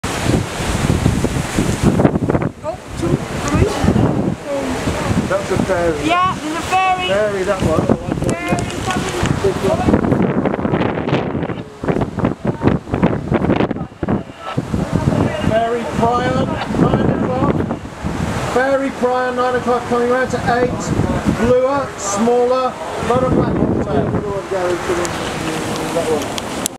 20. Fairy Prion panic
Fairy Prions breed on Marion Island but we only saw them close by to the colonies.  Not an easy bird to see in South African waters.